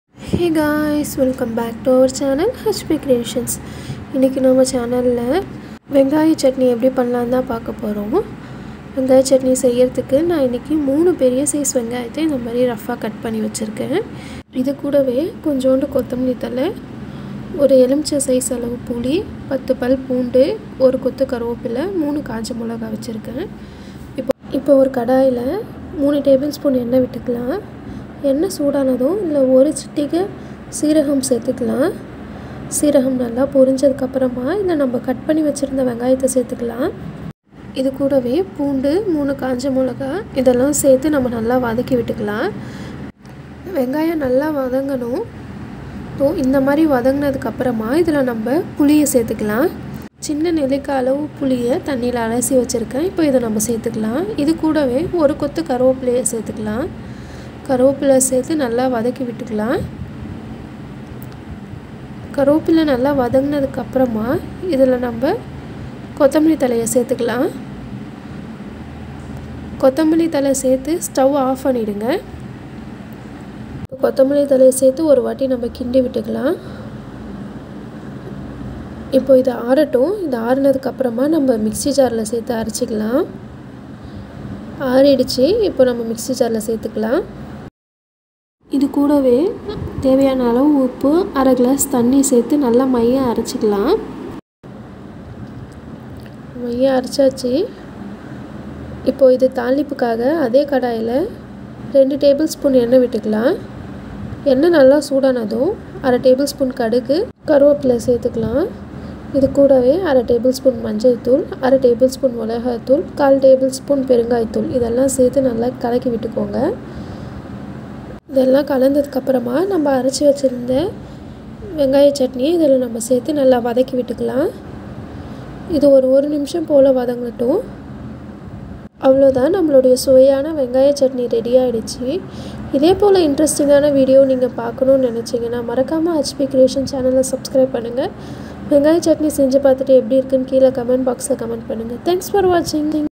onion chutney recipe | வெங்காய சட்னி | hp creations
This video is about how to make onion chutney✨

Ingredients required for onion chutney 👇

dry chilli - 3 no
coriander leaves
curry leaves
garlic - 10 cloves
Tamarind - small amla size
oil - 2 tbsp
cumin seeds - a pinch

For tempering

Mustard - ½ tbsp
curry leaves
Asafoetida -¼ tbsp